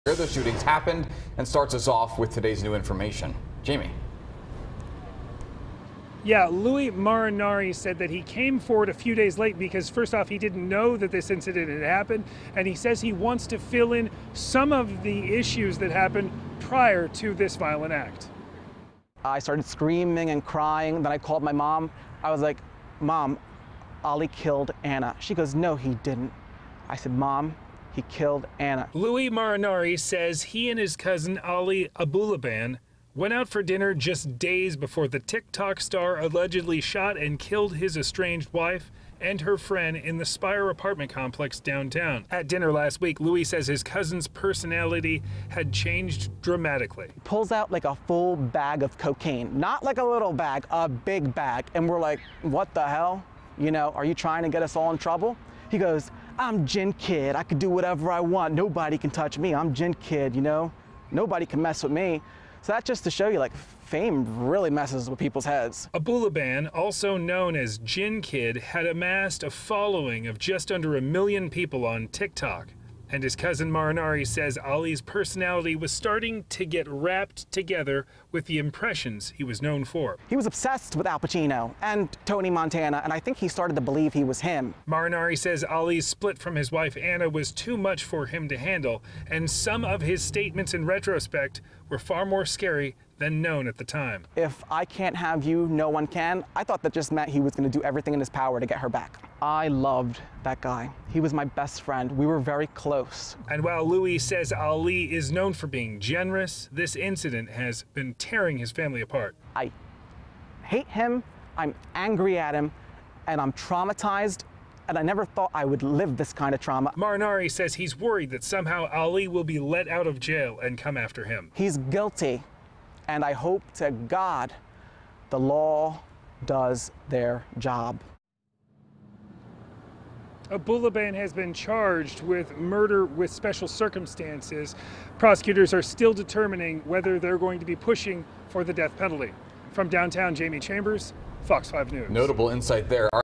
Cousin Of TikTok Star Accused In Double-Murder Speaks Out
The cousin of a TikTok star accused in a double-murder at an East Village apartment last week says his family member's fame and failing marriage had contributed to a major shift in his personality.